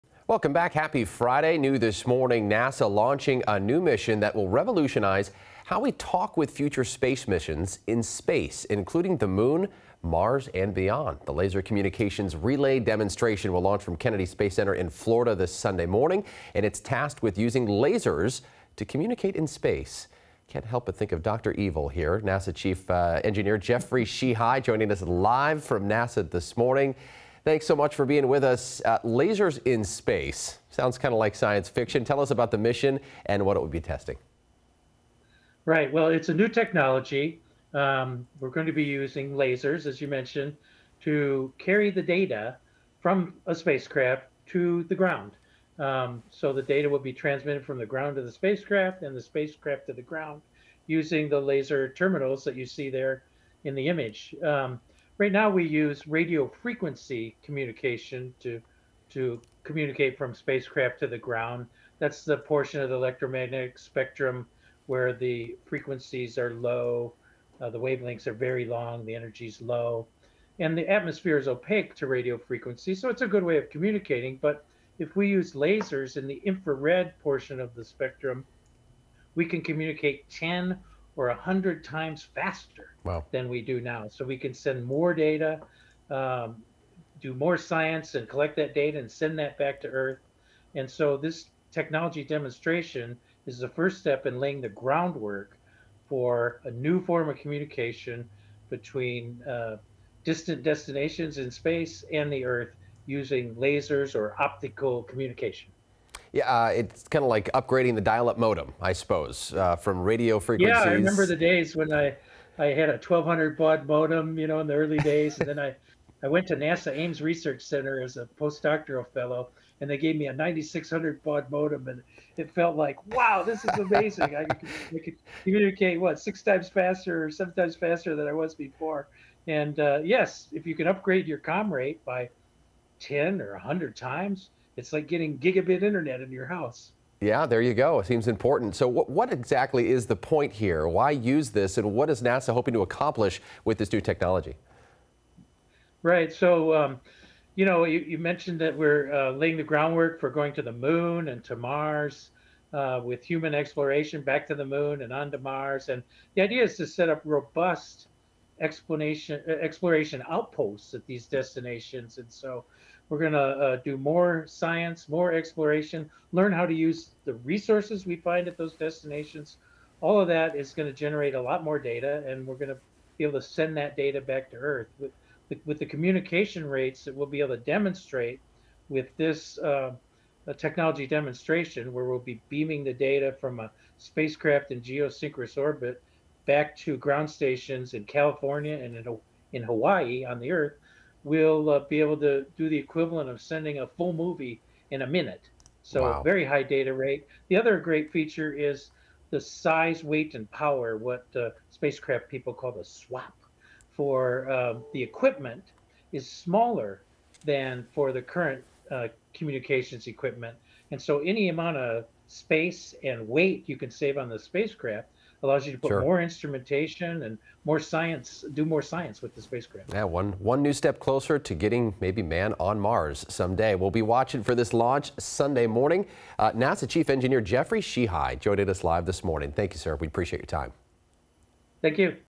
NASA launching new laser communication system
NASA will be launching a new laser technology that will allow communication between the ground and an orbiting spacecraft